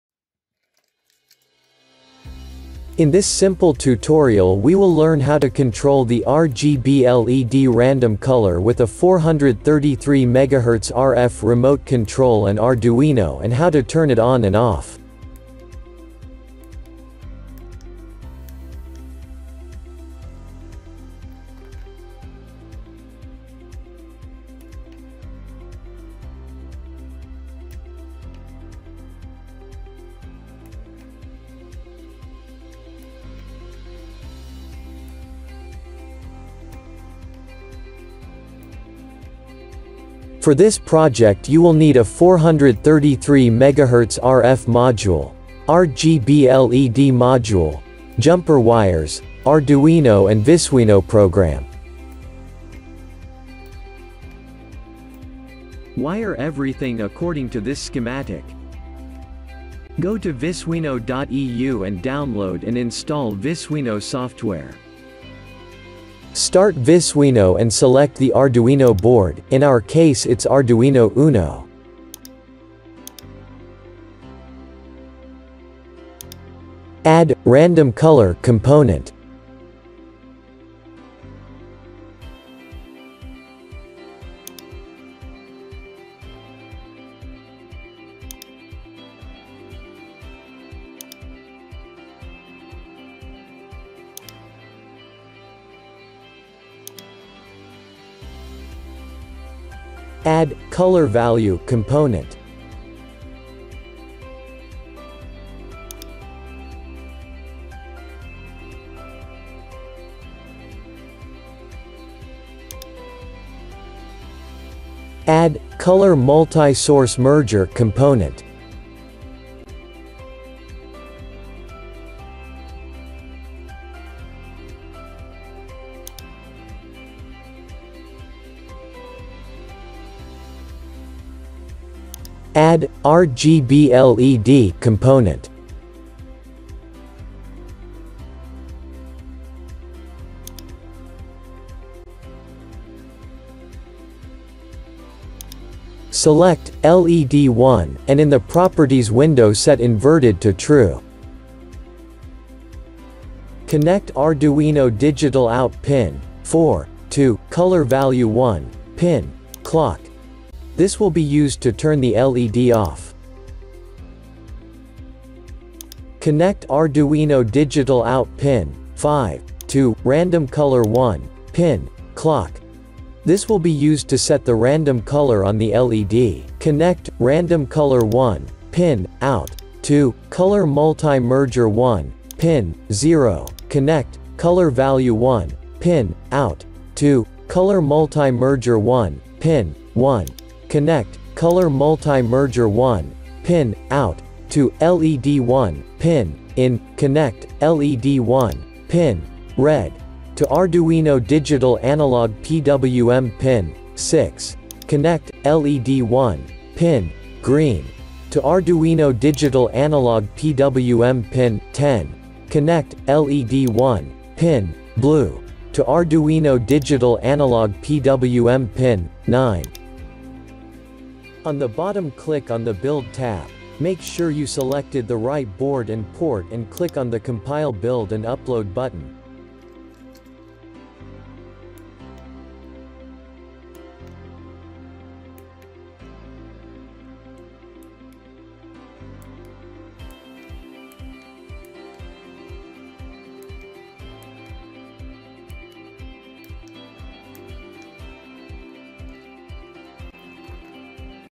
RGB LED Random Color With 433MHz RF Remote and Arduino
In this simple tutorial we will learn how to control the RGB LED Random color with a 433MHz RF Remote control and Arduino and how to turn it ON & OFF.